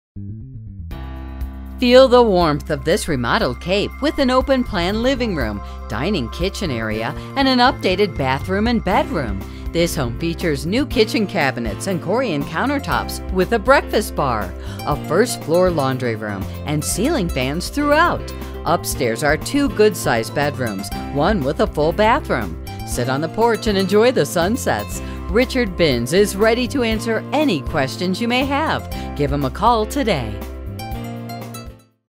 12 Cherry Street Windsor Locks, CT | ColdwellBankerHomes.com
12 Cherry Street, Windsor Locks, CT

Feel the warmth of this remodeled cape with an open plan living room, dining kitchen area with updated bathroom and bedroom. New kitchen cabinets and Corian counter tops with breakfast bar. Two zone heat and air conditioning provided by mini split system. First floor laundry room. Ceiling fans throu